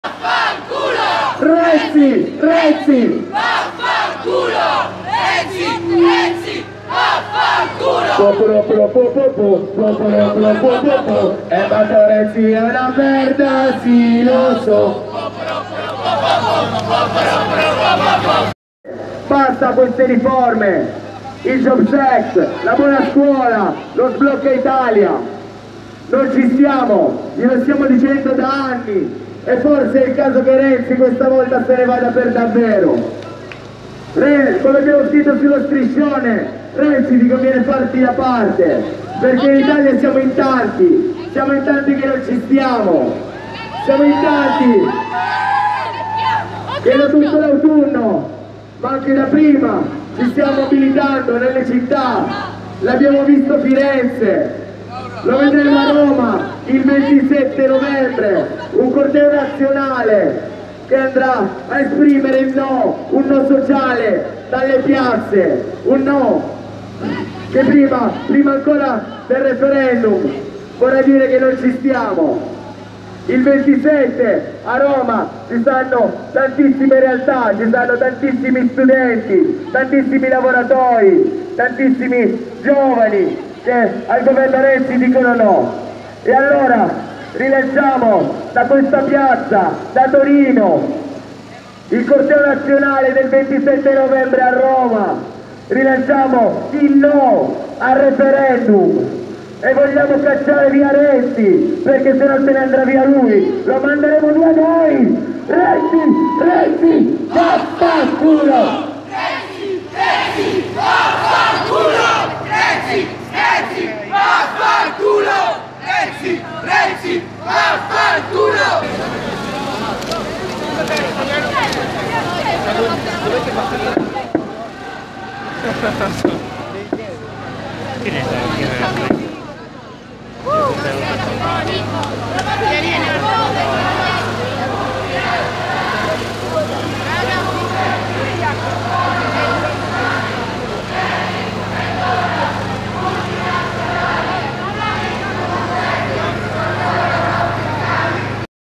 Corteo studenti Torino 17 novembre 2016 - NET
Corteo Studentesco - 17 novembre Pagina evento Facebook - "A Torino gli studenti e le studentesse delle scuole superiori hanno deciso di scendere nuovamente in piazza.
Già il 7 ottobre in molti abbiamo gridato a gran voce che questa idea di scuola non ci piace; tutti assieme abbiamo detto NO alle scuole che crollano in testa agli studenti, abbiamo detto NO ad un’alternanza scuola-lavoro senza garanzie formative dove si è sfruttati, abbiamo detto NO alle deleghe in bianco che mineranno sempre di più la già disastrata istruzione pubblica, abbiamo detto NO ad una scuola escludente fatta per pochi, abbiamo detto NO ad una scuola costosa, antidemocratica, di bassa qualità e abbiamo detto NO a tutte le politiche del governo Renzi e alla riforma costituzionale, simbolo del potere dei pochi. "